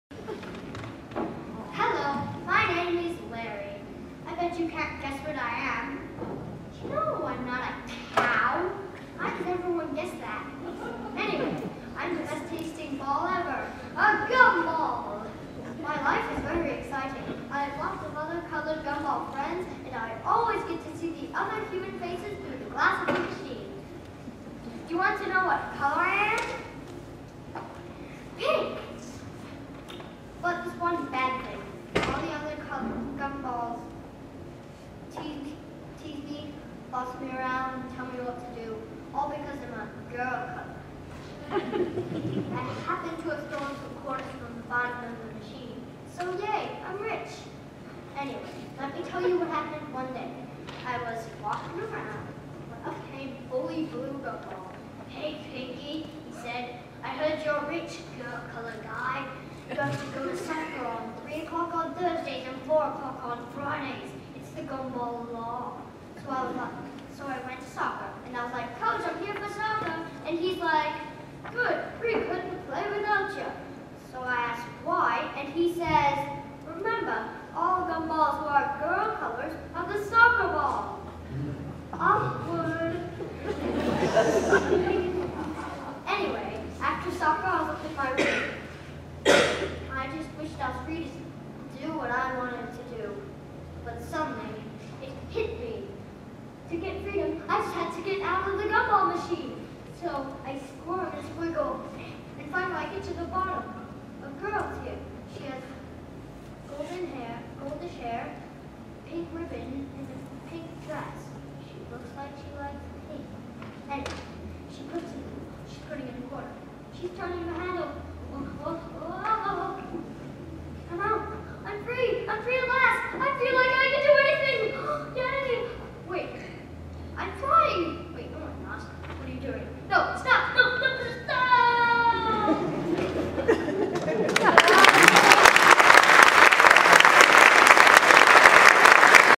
PHCT Curtain Call Kids 2013
Aiden Pasha performs a skit he wrote himself called "Gumball Monologue"